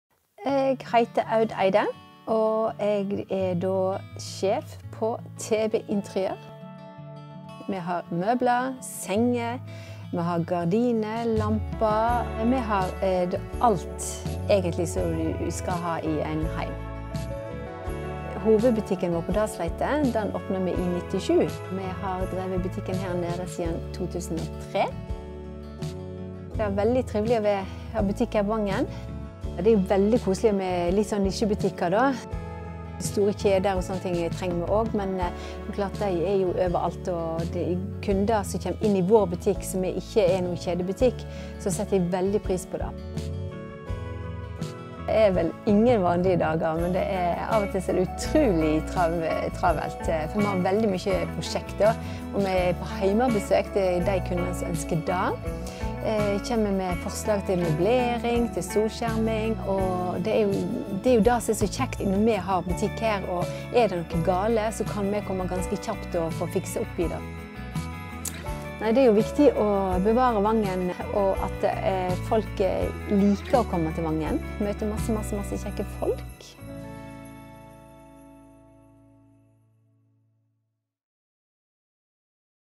Vossavert - TeBe
TeBe er ein møbel og interiørbutikk på Voss. Sjå introduksjonen deira her for å bli betre kjent med dei.